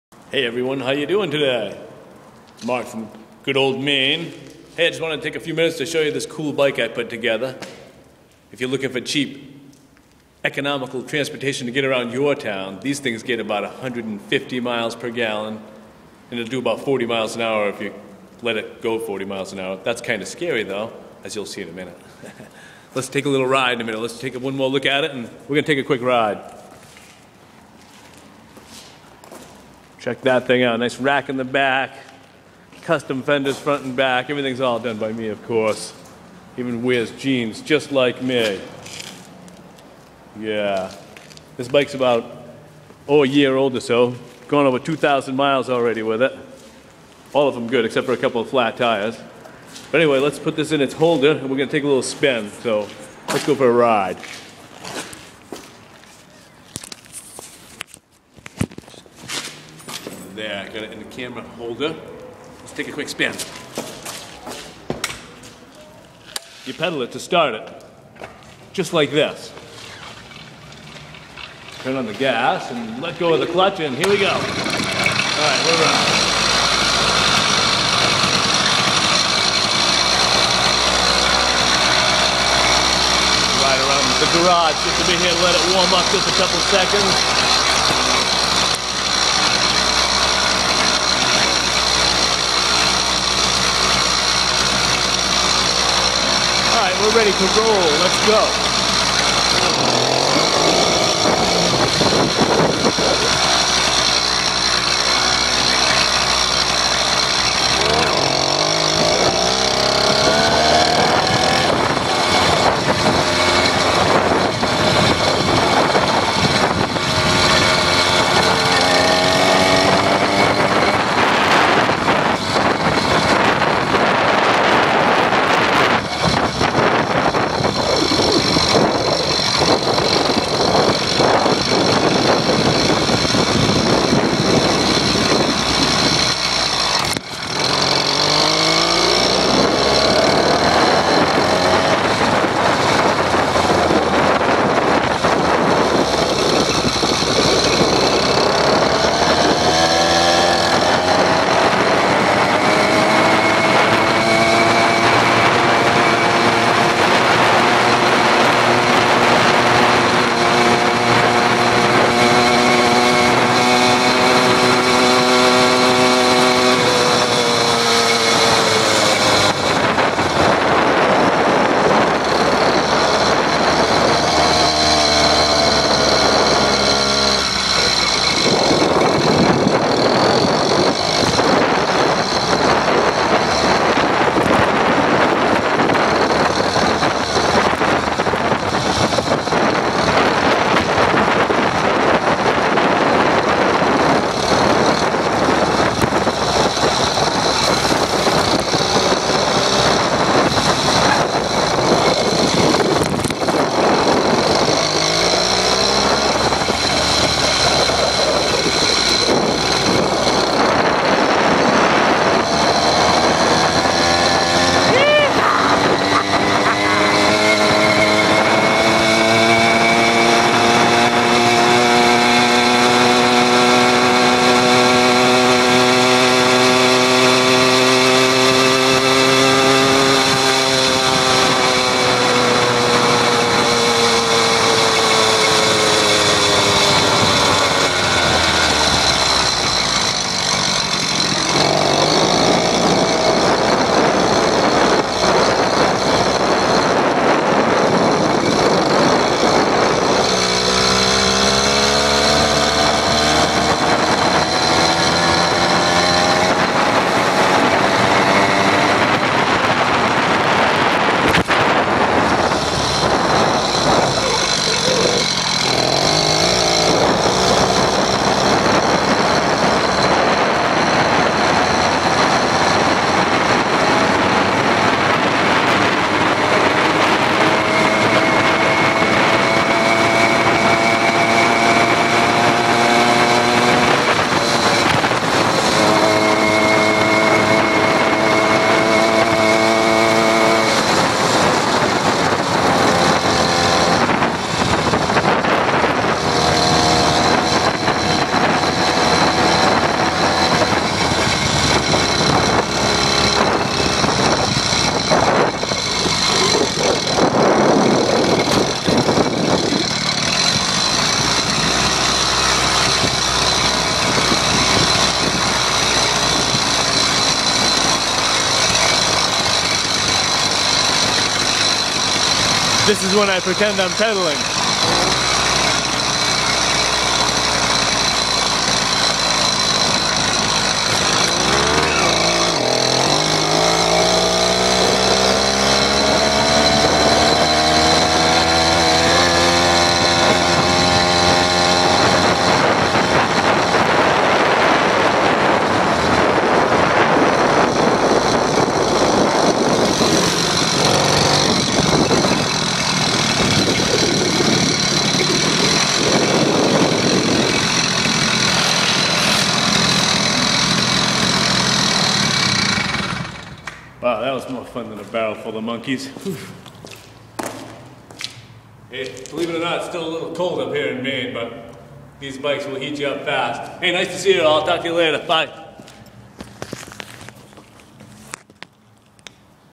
Motorized bicycle get's 150 MPG
Easy to build gas powered bike can go 150 miles on a gallon of gas speeds up to 40 mph